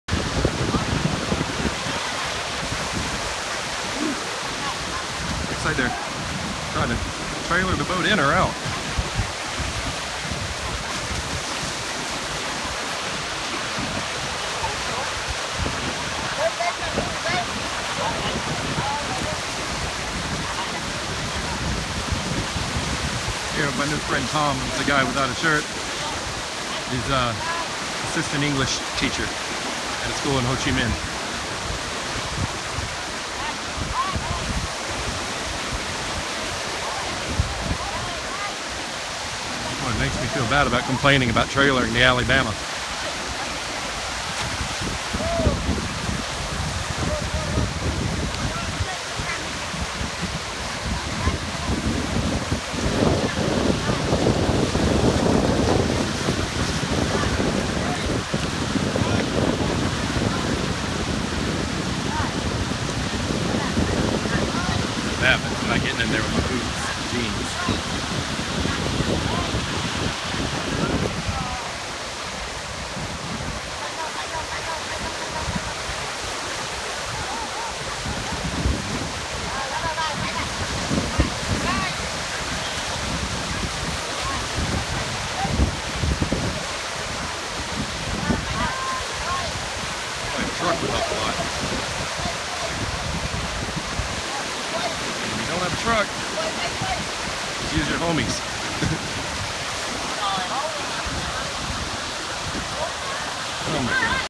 Motorbiking Vietnam - Getting a boat out of water in Long Hai
Day 2 of TheGreatSchlep2016

I stopped off for breakfast at the small beach town of Long Hai, Vietnam on my way from Vung Tau to Mui Ne.  It's a weekend get away spot for the emerging middle class of Saigon.  I watched this fishermen trailering their boat by hand!  I wanted to help them, but I was wearing jeans and boots!

See & read more about my epic motorbike road trip from Saigon to Hanoi